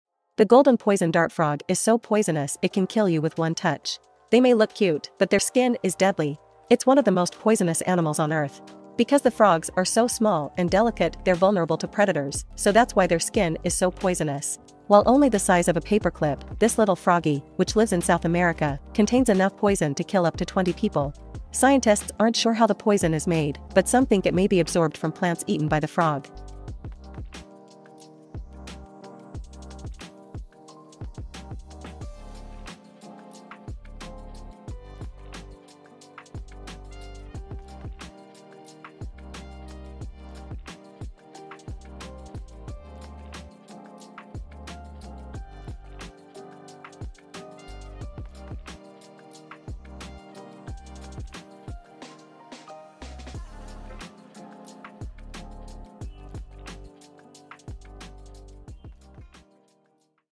🐸 Curious Facts About The Golden Poison Dart Frog | Satisfying Bead Pattern
Hey everyone! In today's video, I will be showing you a fuse bead design of a blue poison dart frog along with some curious facts! Did you these facts about the Golden Poison Dart Frog? ♡